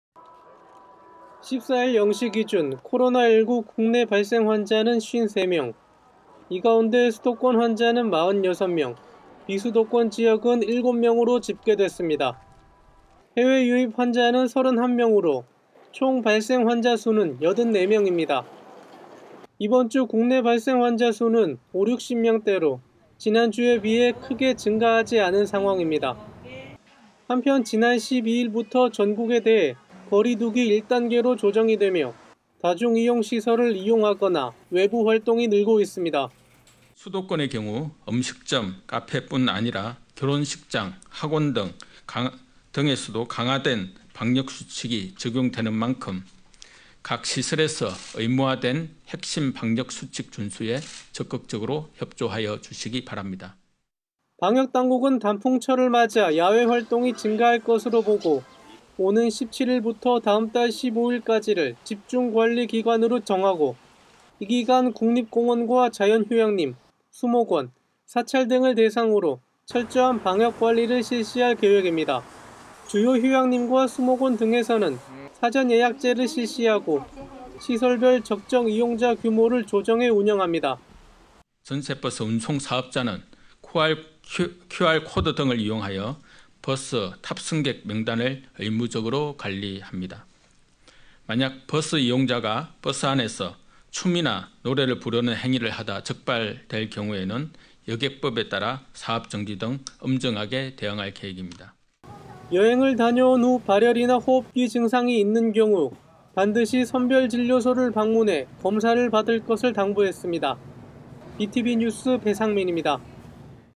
[인천]코로나19 확진자 84명…다중이용이설 방역 강화 /SK브로드밴드 뉴스
코로나19 확진자가 84명으로 두 자릿수를 유지하고 있습니다.
이번 주부터 사회적거리두기 완화로 다중이용시설과 단풍철 야외활동이 증가할 것으로 예상되는데요.
방역당국은 대책 마련에 분주한 모습입니다.